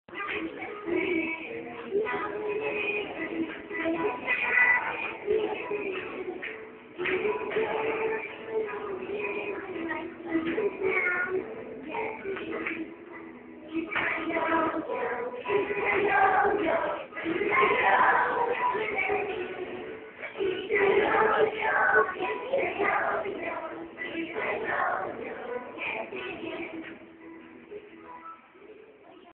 la clase del ticher
aki haciendolos kantar a los chukis